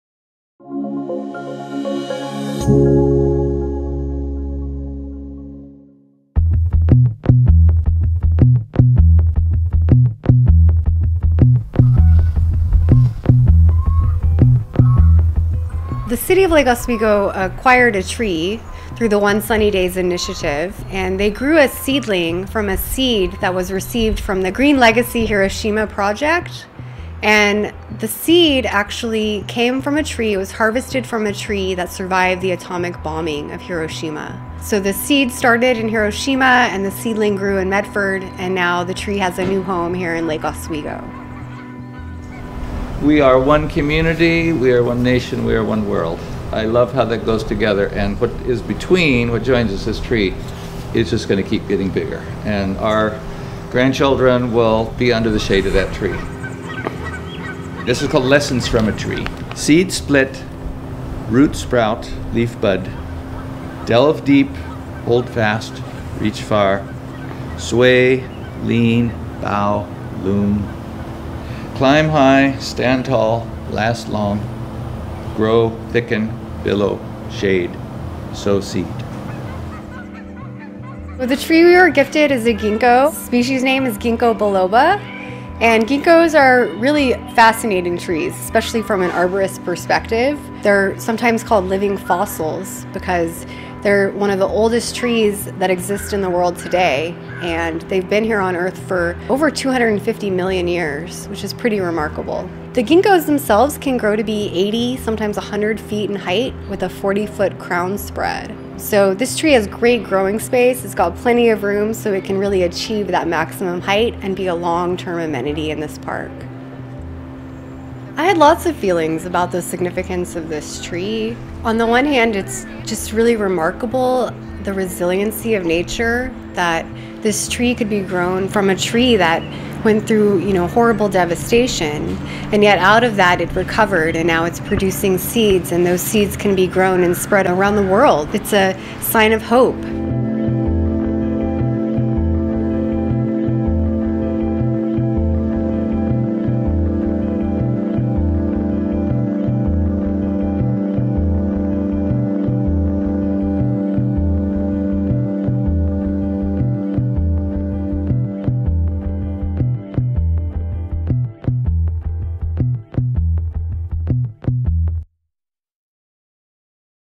Foothills Park Extraordinary Tree Planting, Lake Oswego
Lake Oswego celebrated Arbor Week and National Poetry Month by planting a tree seedling at Foothills Park. The seedling was grown from a seed of a tree that had survived the atomic bombing of Hiroshima in 1945. It was brought to Lake Oswego by the One Sunny Day Initiatives Program through the Green Legacy Hiroshima Project.